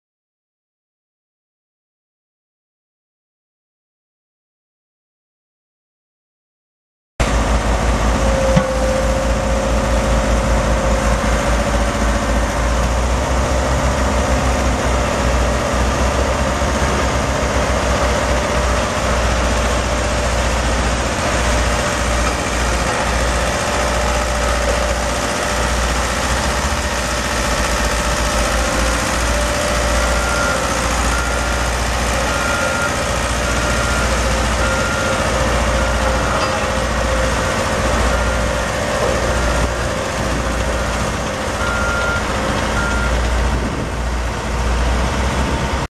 Case 602D smooth drum roller for sale | sold at auction July 30, 2015
Case 602D smooth drum roller
2,402 hours on meter
2,236 hours on other meter
Duetz F4L912 four cylinder diesel engine
73 HP
Non-operational
Hydrostatic transmission
OROPS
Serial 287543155
Vandal cover
Independent drum controls
68.5"W front drum
Front drum scrapers
Alliance 13.6-28 rear tires
Engine issues
One tire flat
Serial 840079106

It was located in Bridgeton, MO, and we sold it to the highest bidder for $4,510.